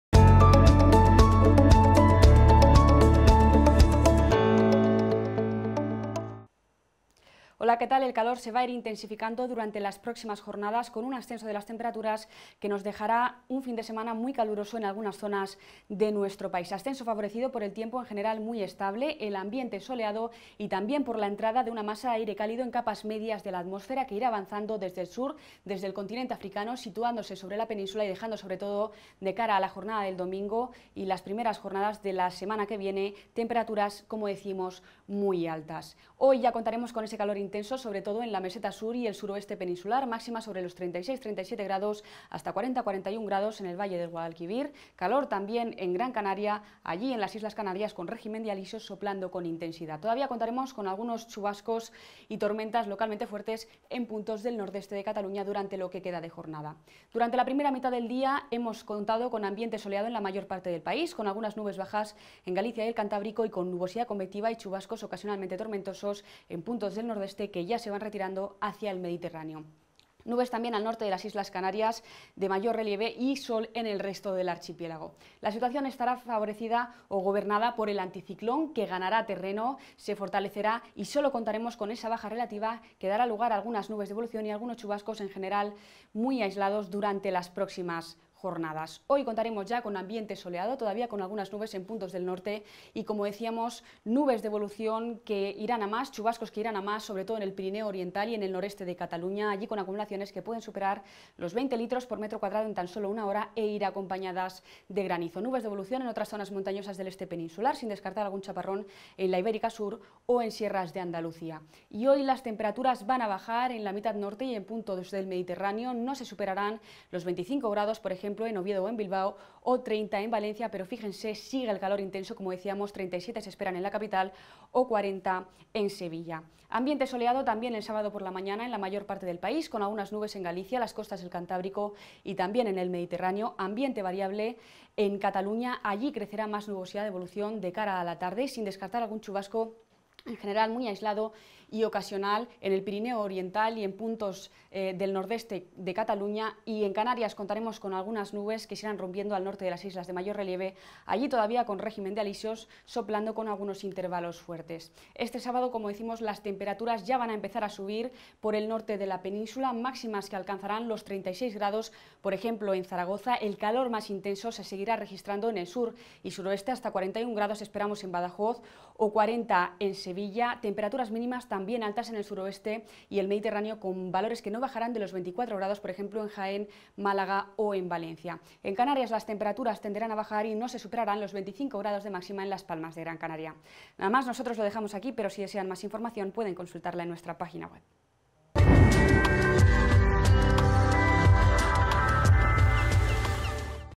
El tiempo de AEMET 24-07-2020
24 de julio de 2020
Fenómenos significativos
Posibilidad de alguna tormenta localmente fuerte en el Pirineo Catalán. Temperaturas altas en el interior de la mitad sur peninsular y puntos de Canarias.
 25 de julio de 2020
Fenómenos significativos
Temperaturas altas en el interior de la mitad sur peninsular y en el valle del Ebro.